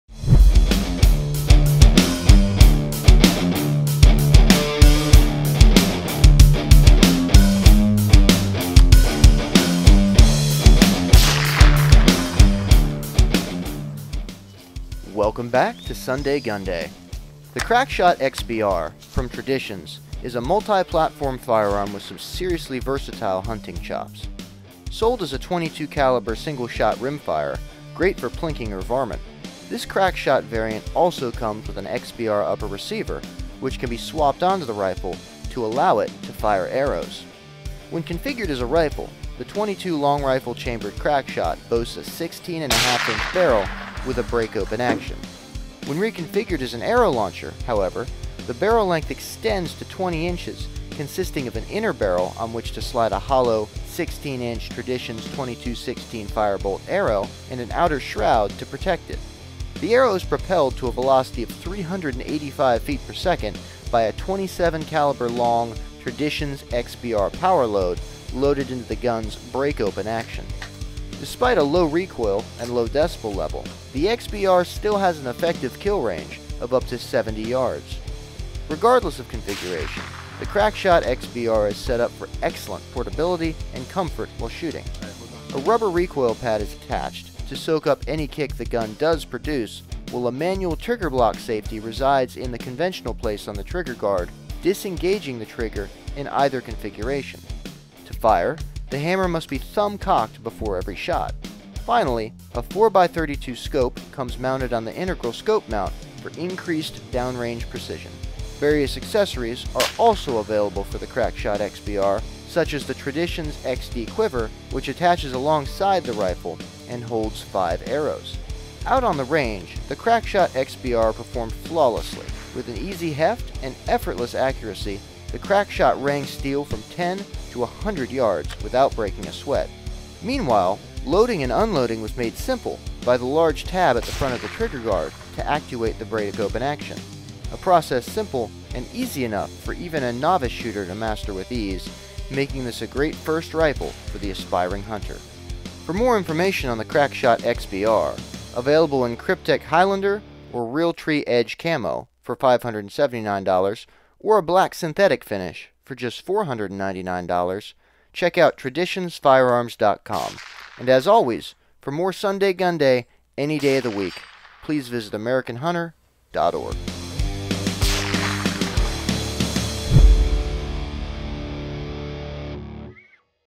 SundayGunday: Traditions Crackshot XBR
Get a closer look at the Crackshot XBR, from Traditions, the latest addition to our SundayGunday series.